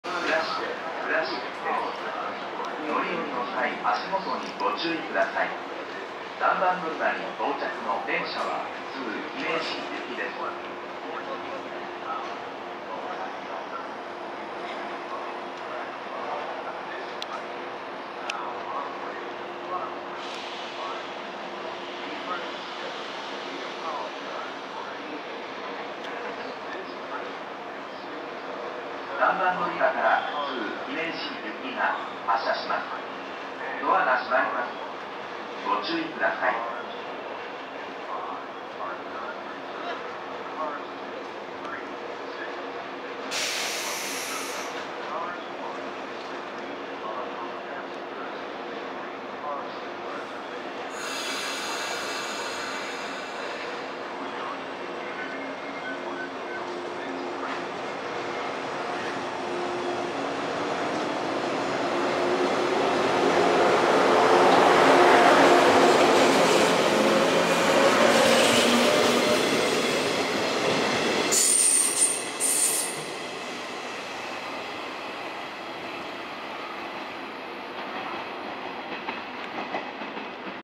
【数少ない姫路行】115系普通姫路行発車（倉敷駅3番のりば）
倉敷駅を発車する、数少ない115系普通姫路行4両編成です。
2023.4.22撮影